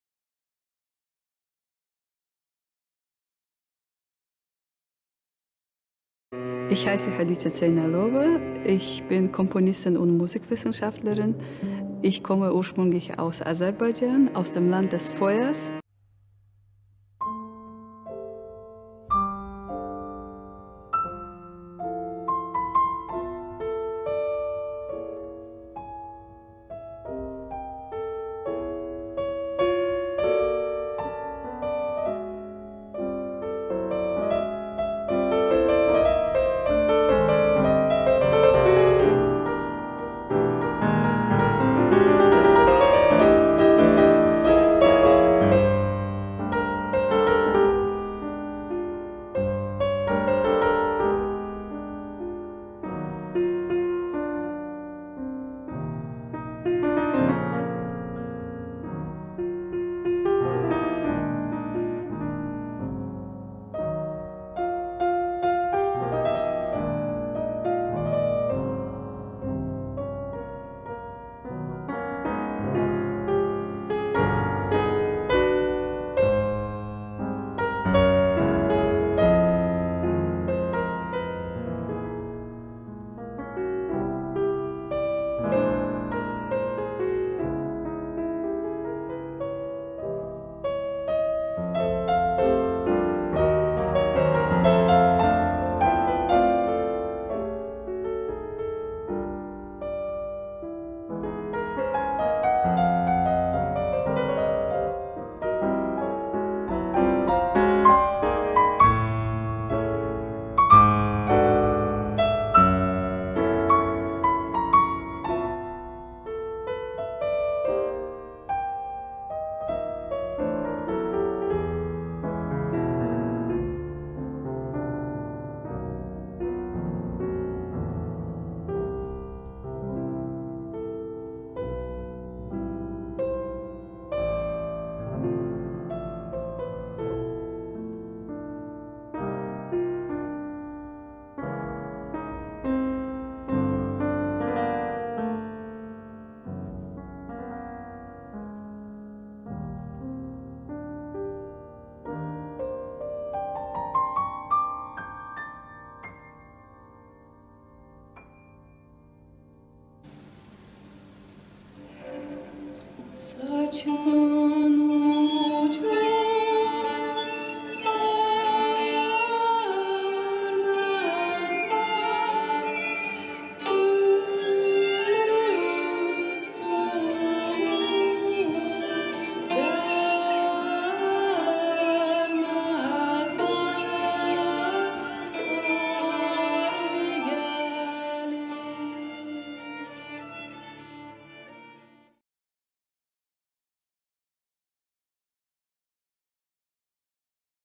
Hazırda Almaniyada yaşayan Xədicə Zeynalova, peşəkar bəstəkar kimi fəaliyyət göstərir.
Hazırda Almaniyada yaşayan Xədicə Zeynalova, peşəkar bəstəkar, pianoçu, orqançı, musiqişünas və pedaqoq kimi fəaliyyət göstərir.